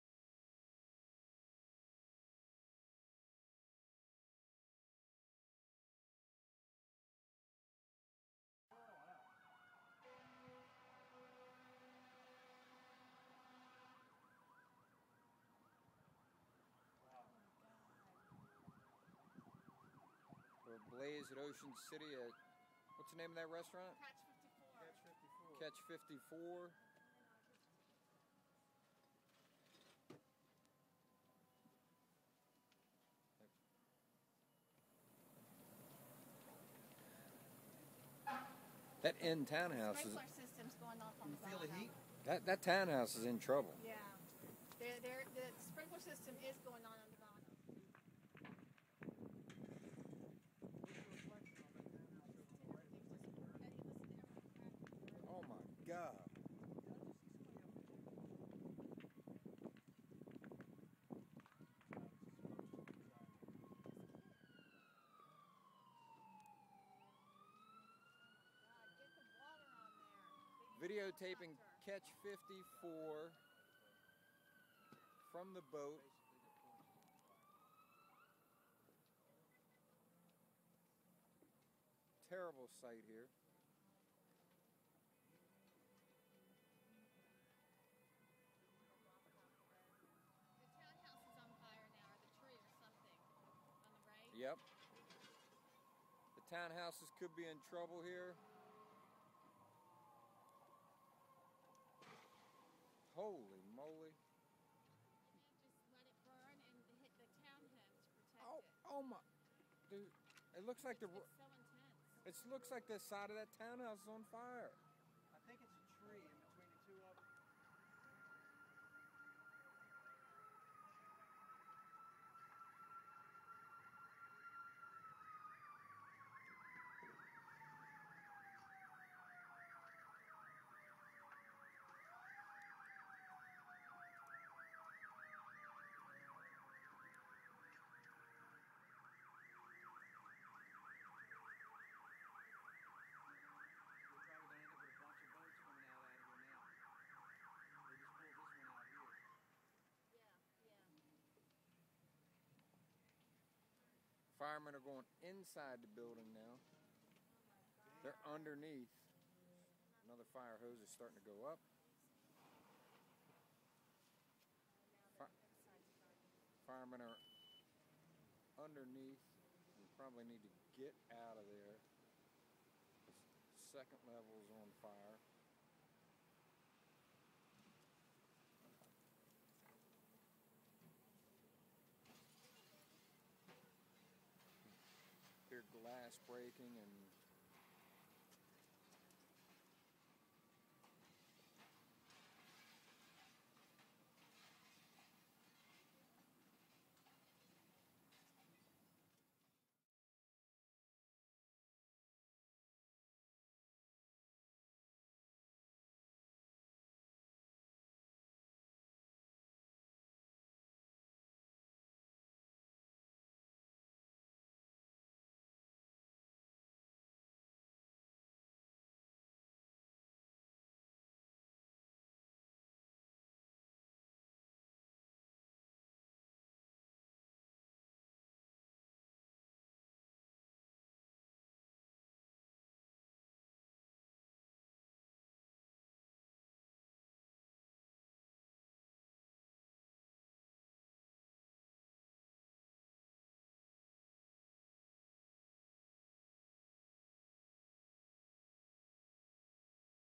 Catch 54 Restaurant in Fenwick Delaware Burns - OC Md News
Fire caused major damage to the popular Catch 54 Restaurant on Madison Avenue in Fenwick, Delaware on Tuesday Evening, August 2nd, 2011.

All customers were evacuated to safety, and there were no injuries.

Fire equipment from Roxana arrived on the scene at 8:35 PM and were soon supported by Ocean City and Bethany Units.  The fire was under control by 9:15.  Other support arrived from Bishopville, Frankford, Millville and Rehoboth Beach.

The next six weeks are the busiest of the year, so the fire came at a very bad time.  It is estimated that it will be several months before the restaurant reopens.

** Thanks to Scott R. for the raw video and narration ...